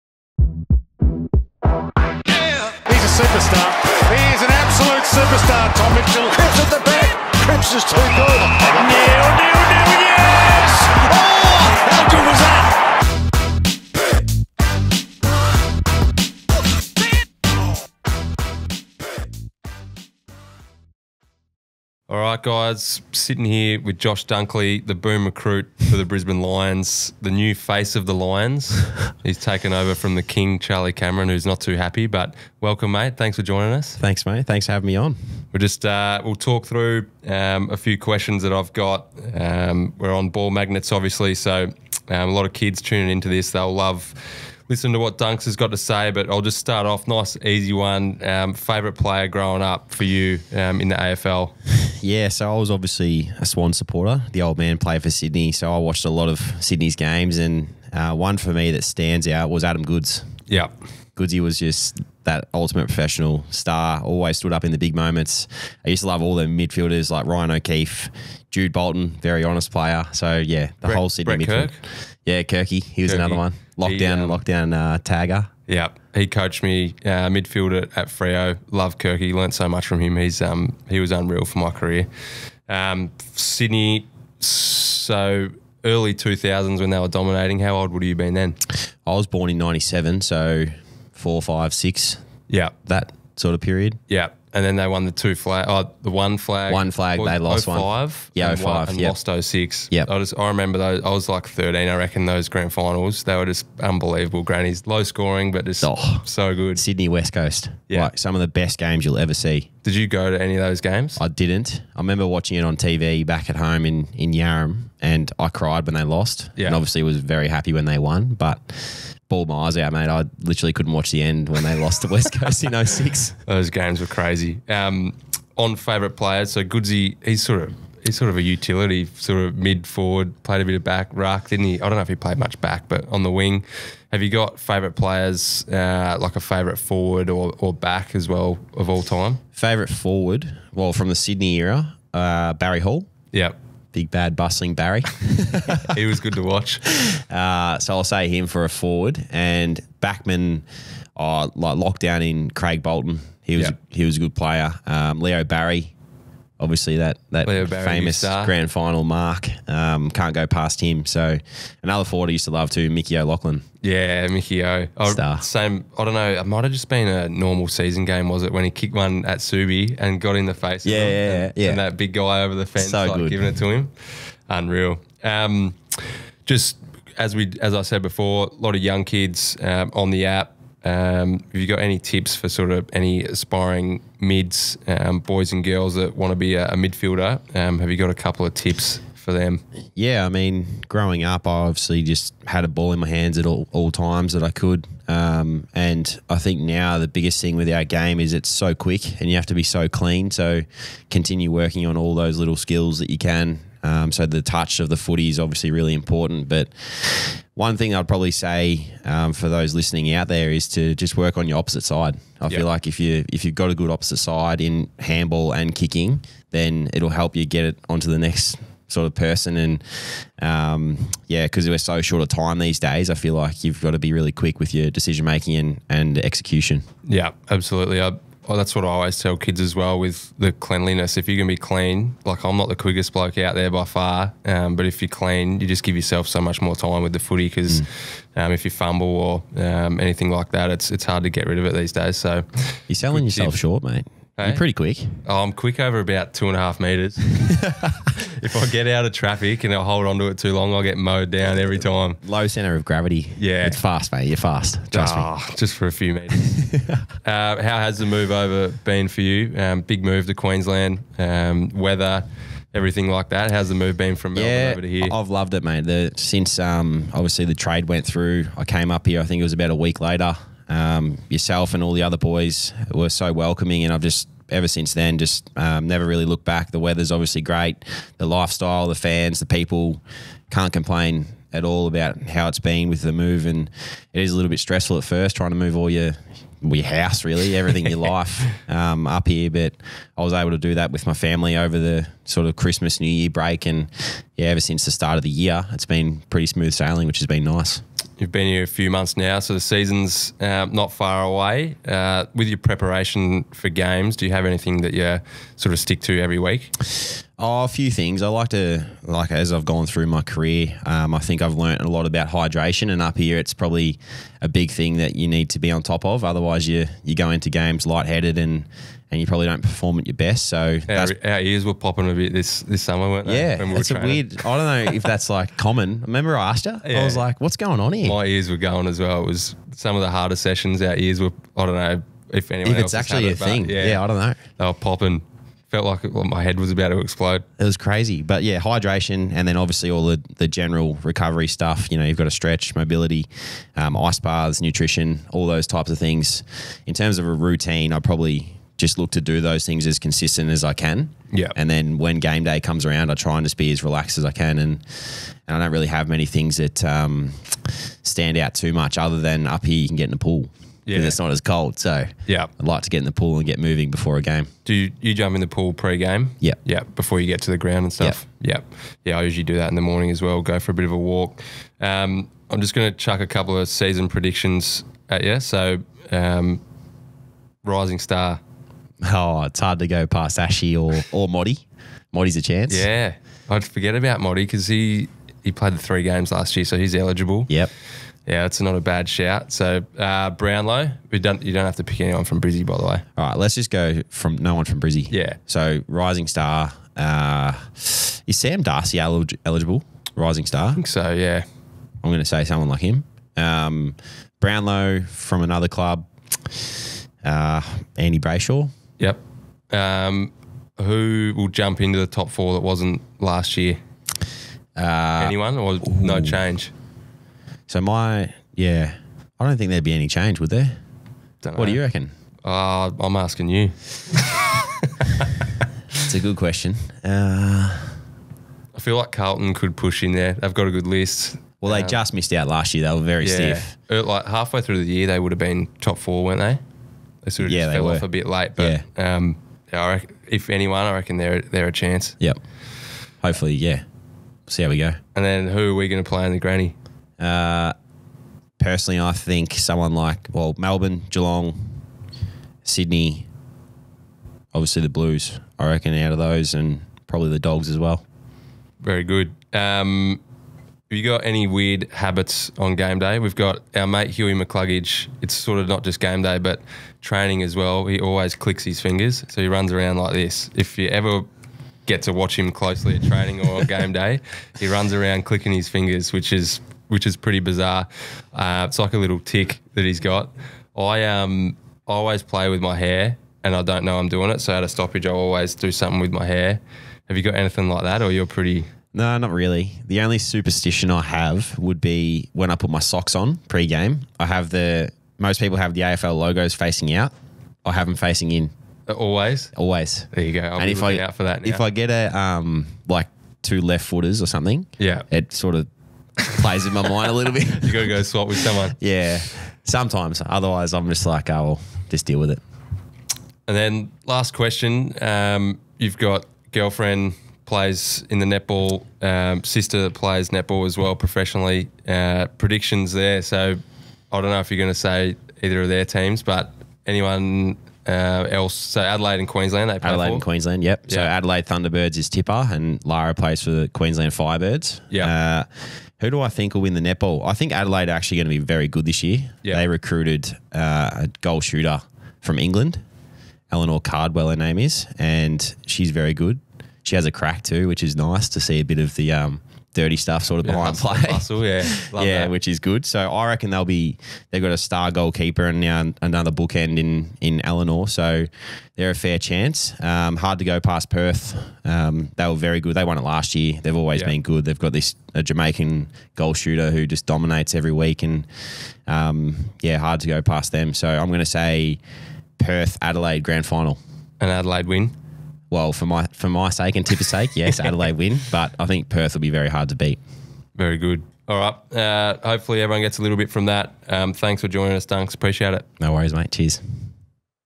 Josh Dunkley On New Club, Season Predictions & All Time Players | Ball Magnets Podcast #17
With Lachie's first time hosting he's joined by new teammate & 2022 Western Bulldogs best & fairest winner Josh Dunkley. Josh covers his AFL journey from a young player, his inspirations, tips and tricks & his 2023 season predictions.